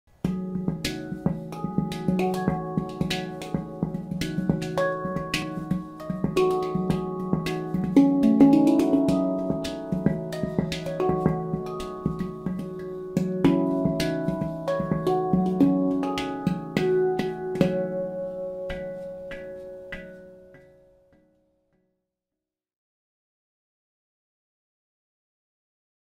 The creating handpan phrase | Improvisation time 33
Can you play the rest of the music?

Challenging myself and creating.
I hope you like what you hear and see.

Pantam : Hoia Pantam
Scale : F# La Sirena 16